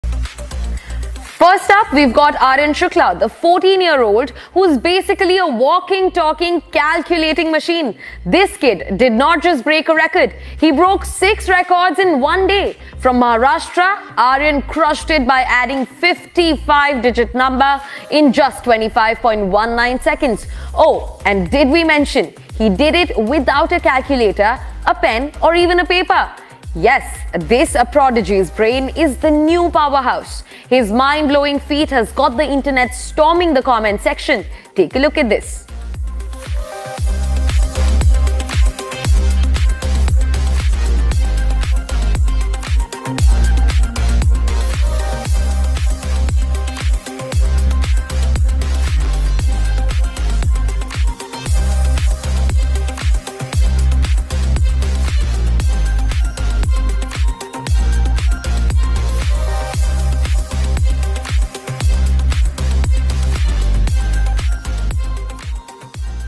Human Calculator Aaryan Shukla from India shattered 6 Guinness World Records in Mental Calculation
Mental Calculation World Champion Human Calculator Aaryan Shukla created 6 New Guinness World Records in the field of Mental Calculation at Dubai in UAE.
Fastest time to mentally add 100 four-digit numbers - 30.9 sec
Fastest time to mentally add 200 four-digit numbers - 1 min 9.68 sec
Fastest time to mentally add 50 five-digit numbers - 18.71 sec
Fastest time to mentally divide a 20-digit number by a ten-digit number (set of ten) - 5 min 42 sec
Fastest time to mentally multiply two five-digit numbers (set of ten) - 51.69 sec
Fastest time to mentally multiply two eight-digit numbers (set of ten) - 2 min 35.41 sec